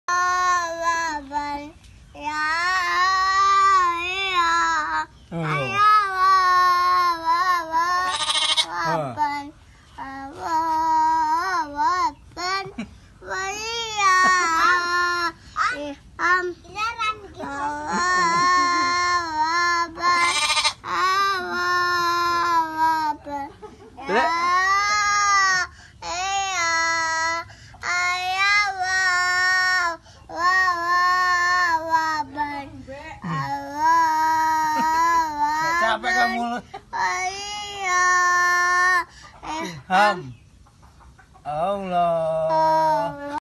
Baby zuqna potong kambing qurban😆
cita-cita mau jadi ustad buat bisa potong qurban